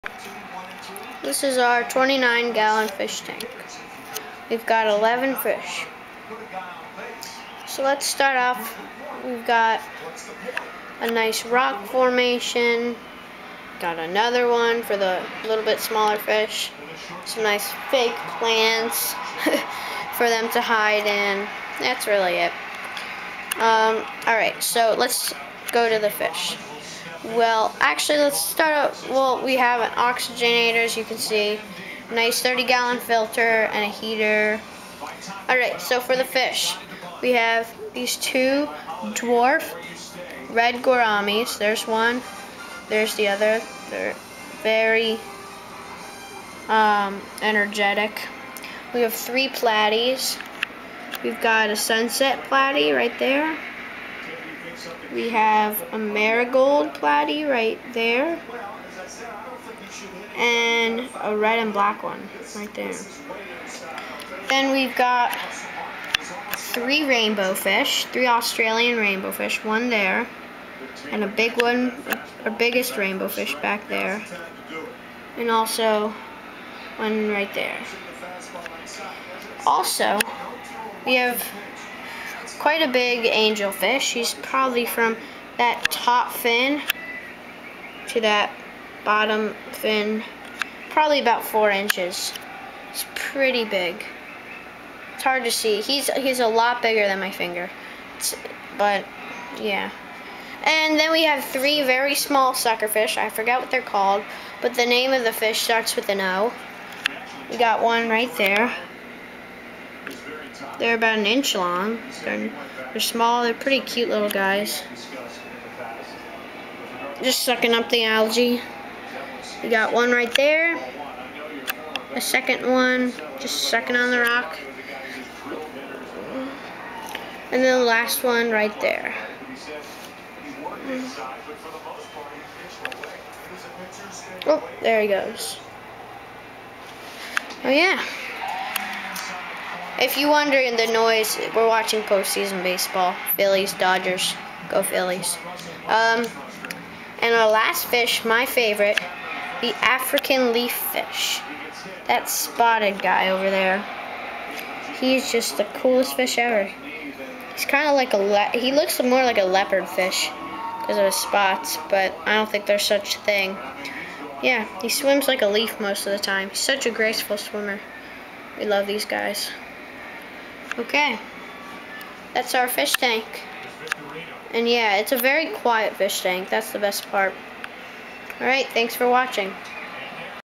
My 29 Gallon Fish Tank
We have quite a nice, clean, and colorful tank. we have a leaf fish, platys, rainbow and angelfish, and 3 small suckers 2 new fish coming soon!!